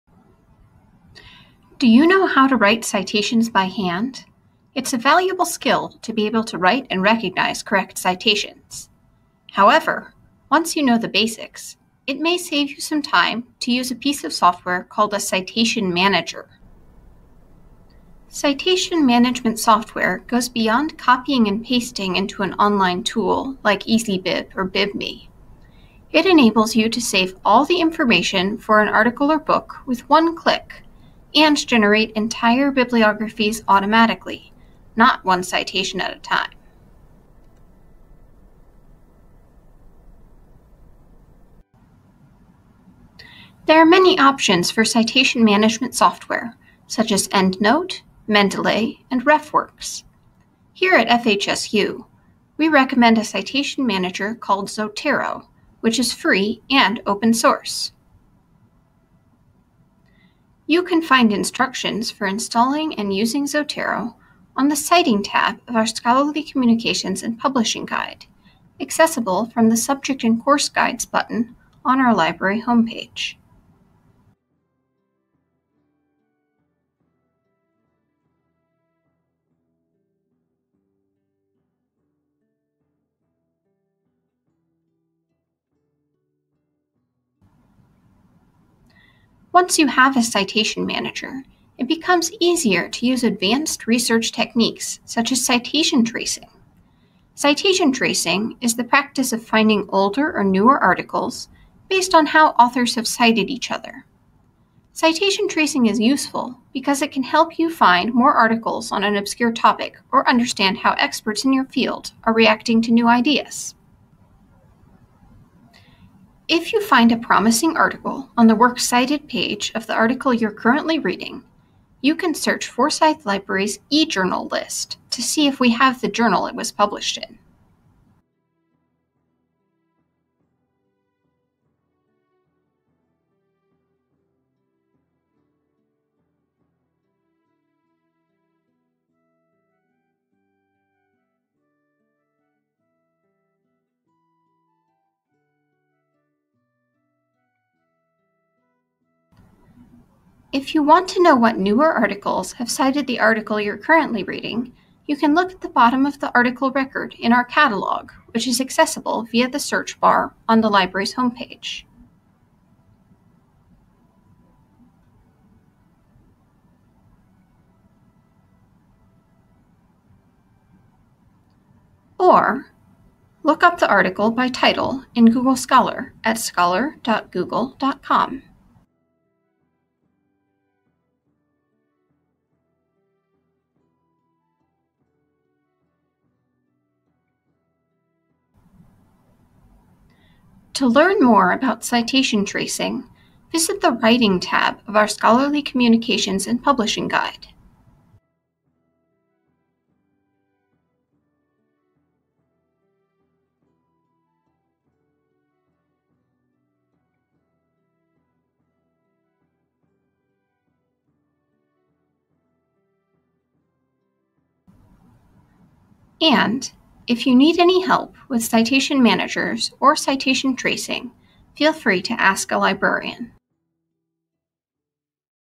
Citation Management and Citation Tracing
This video discusses citation management and citation tracing when searching for sources, including:

 - Citation management software: Zotero
 - Instructions for installing and using Zotero
 - Citation Tracing
 - Search e-journal
 - Using article record
 - Using Google scholar
 - Scholarly Communications and Publishing Guide